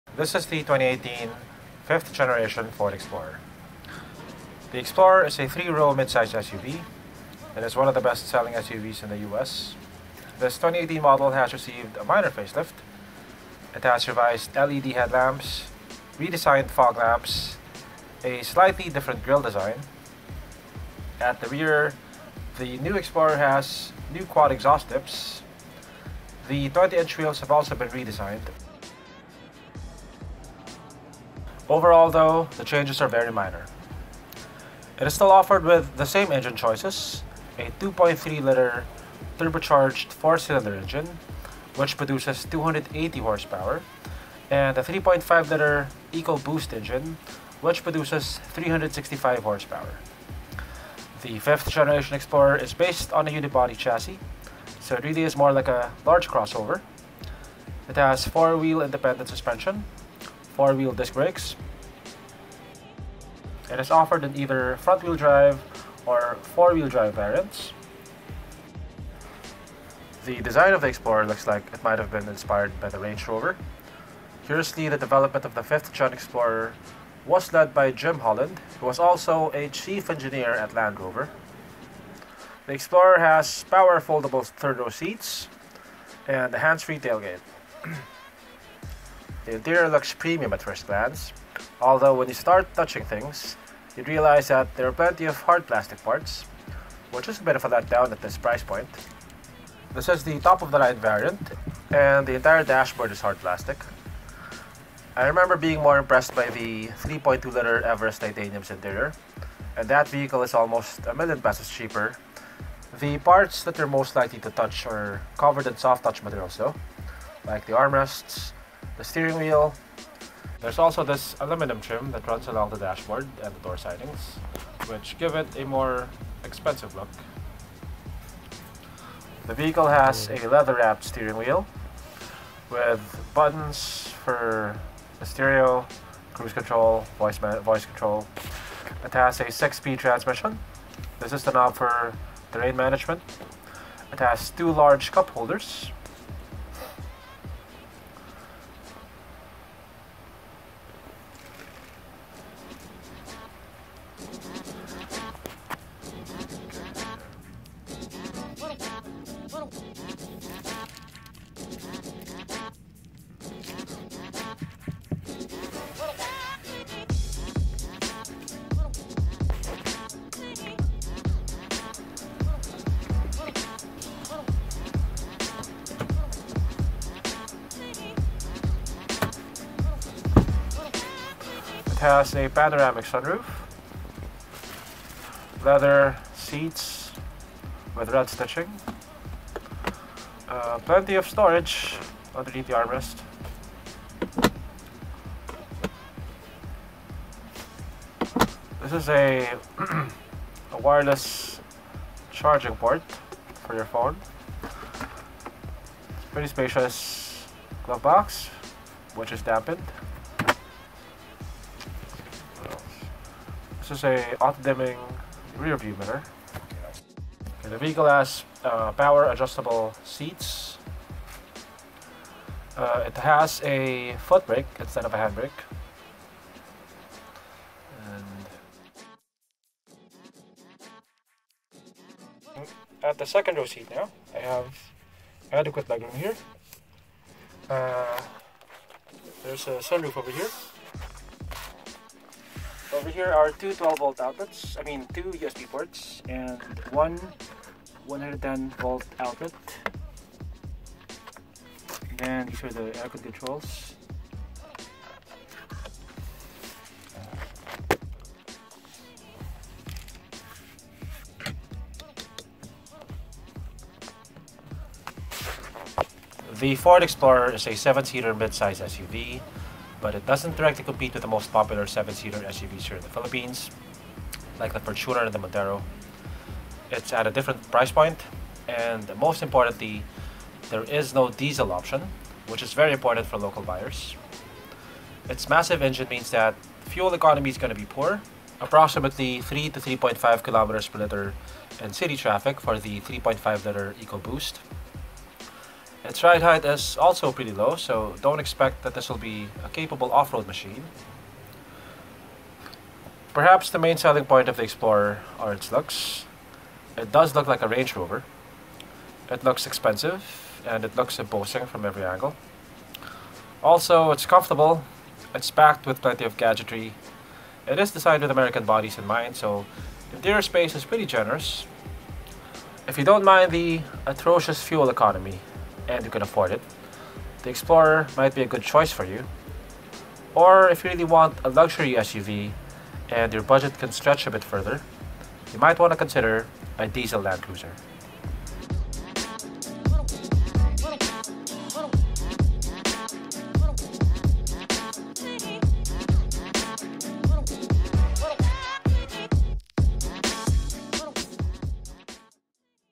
2018 ford explorer 3.5L Ecoboost 4WD "facelifted" video review/vehicle tour
This is a vehicle tour of the facelifted, 2018 Ford explorer 3.5 Ecoboost which sells here in the philippines for 3,178,000 Php or 63,560 USD. The 2.3L variant sells for 2,798,000 Php or 55,960 USD